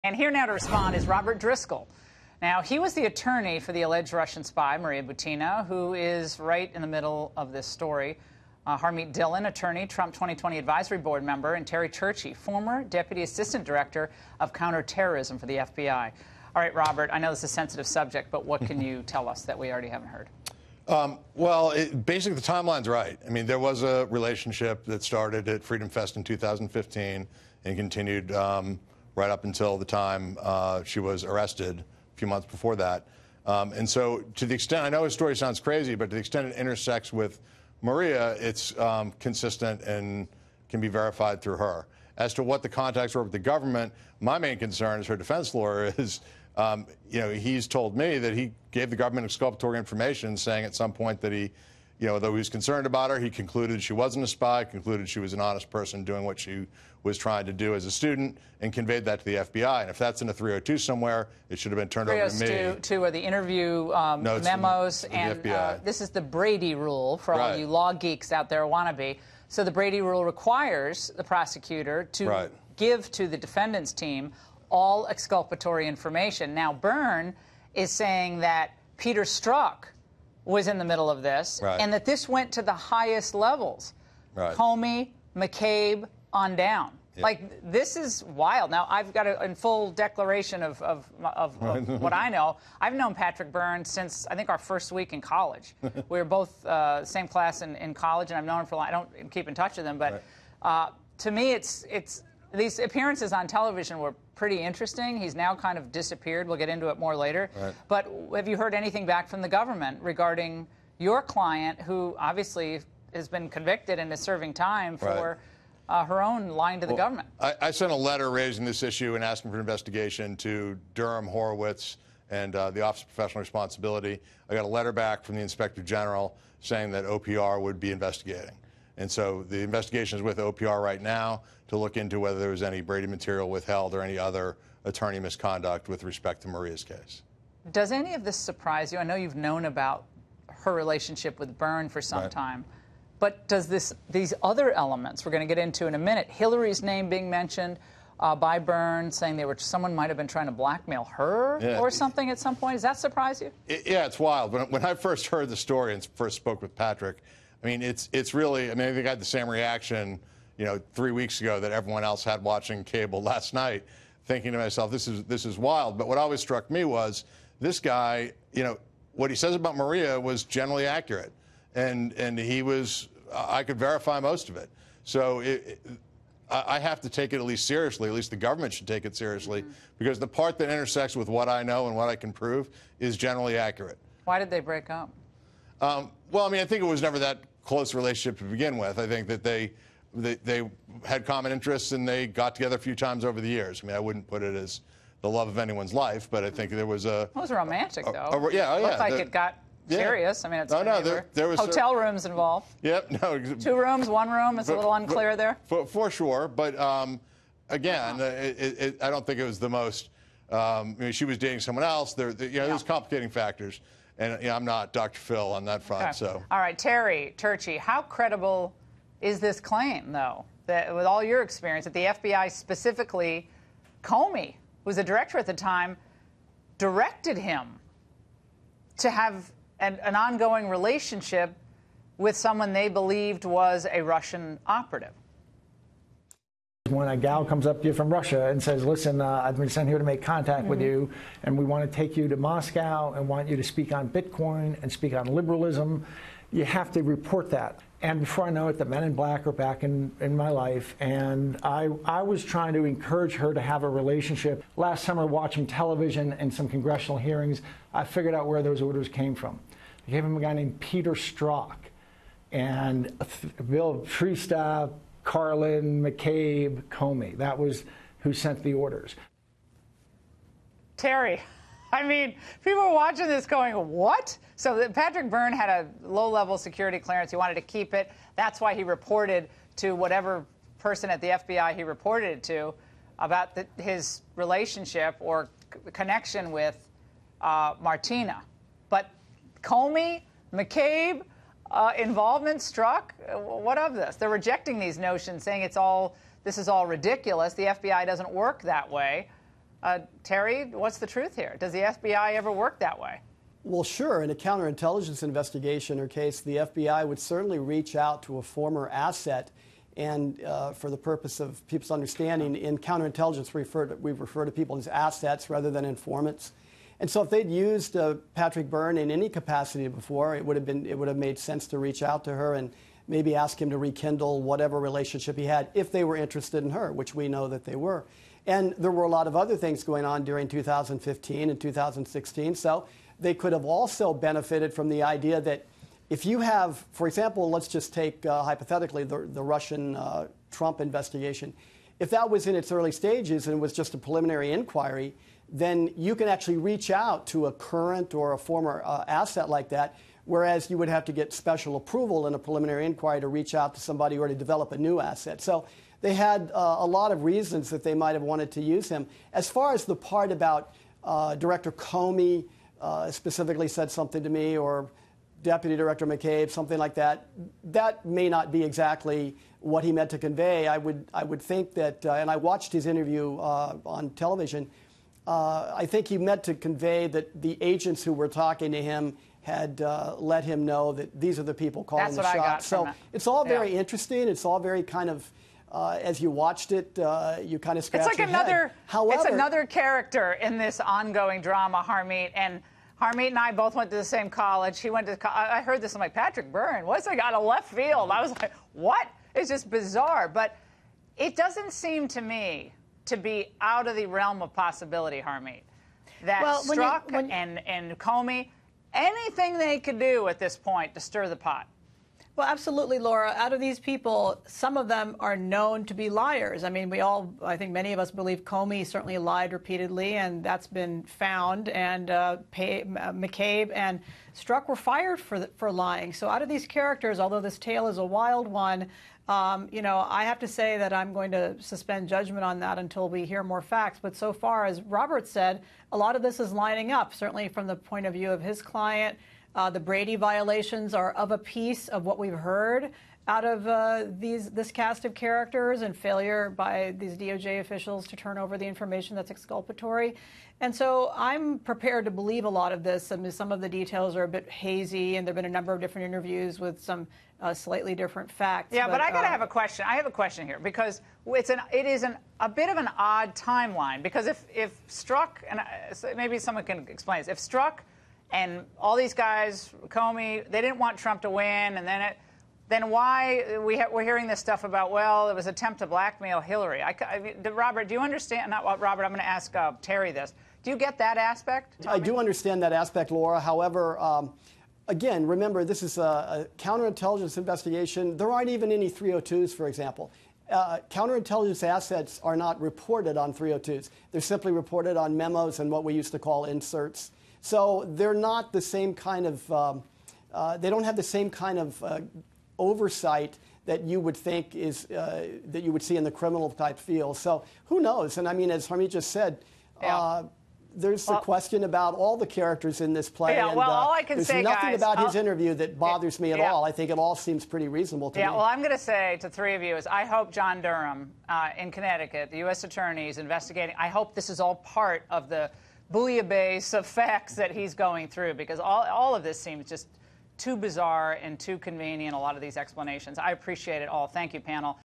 Maria Butina's attorney responds to claims FBI told Byrne to pursue her
Robert Driscoll confirms the former Overstock CEO had a relationship with the alleged Russian spy.